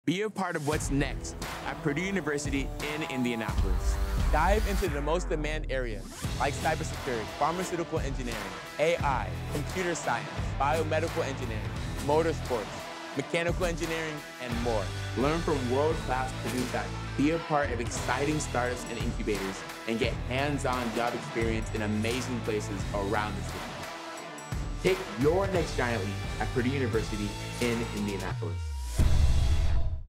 Be a part of what’s next at Purdue University in Indianapolis
It’s time to take your next giant leap at Purdue University in Indianapolis.

Get real-world, hands-on experience and become an expert in critical and emerging STEM fields, from cybersecurity to motorsports engineering.

Work with top employers (located down the street) and connect with the business leaders, researchers and faculty who are providing solutions to today’s toughest challenges.

Be at the forefront of innovation — again and again and again.